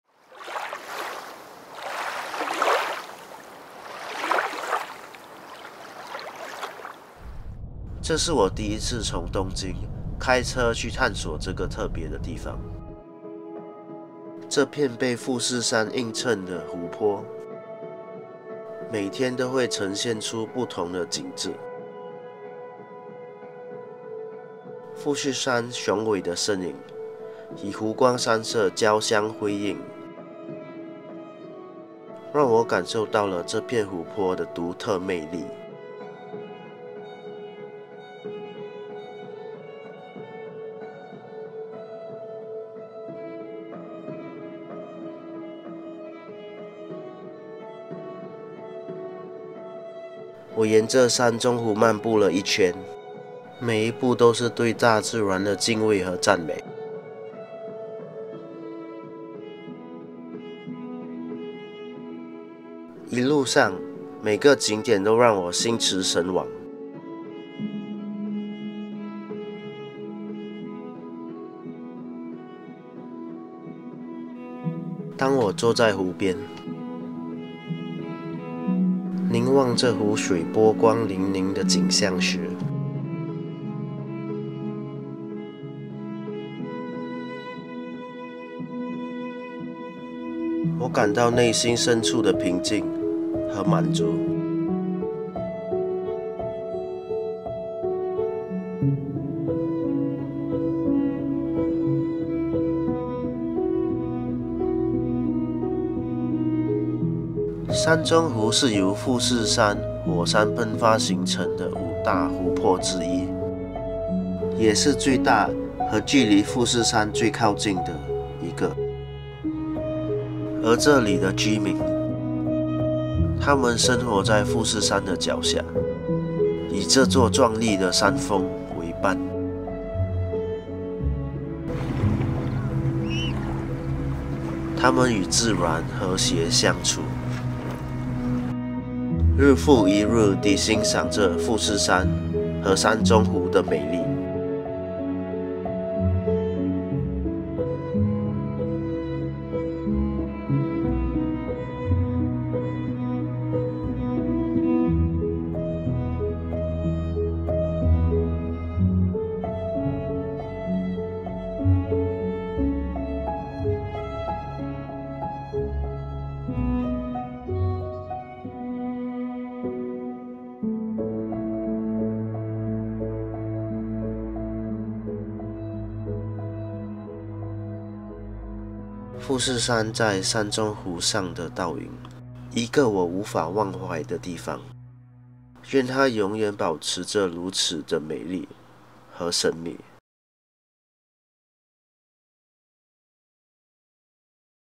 难忘的美丽：探索山中湖
从宁静的湖水倒映着富士山的雄伟轮廓，到充满自然奇观的生机勃勃的周边环境，山中湖为您提供了一窥日本风景如画的迷人魅力。随着我们沿岸探索，深入探讨这个迷人目的地的丰富历史和文化意义，您将发现为什么山中湖常被自然爱好者和摄影师誉为隐藏的宝藏。

Filmed on 2-3 Nov  2023